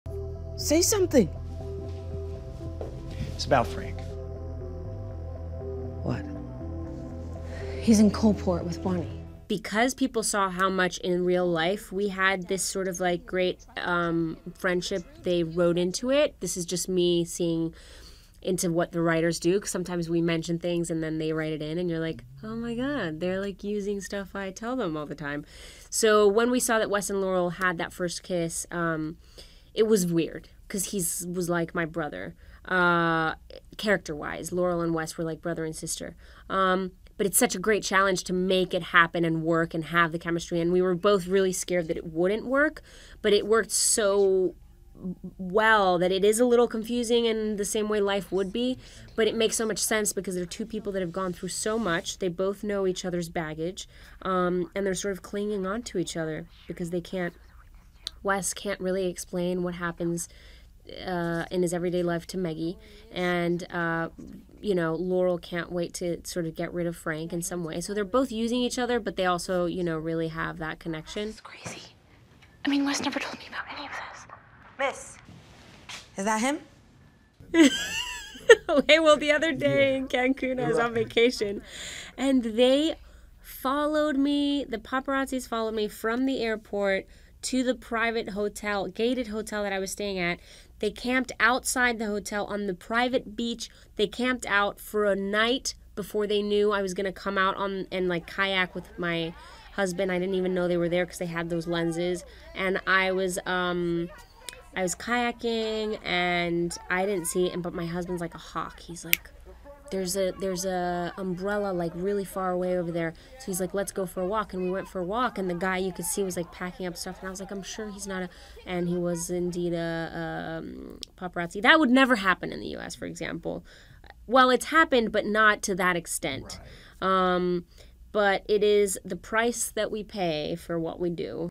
Karla Souza on fame in Mexico versus the U.S.
'How to Get Away with Murder' actress Karla Souza, talks about her character Laurel's budding relationship with Alfred Enoch's character, Wes. She also shares what it's like to be one of Mexico's biggest stars. (Oct. 27)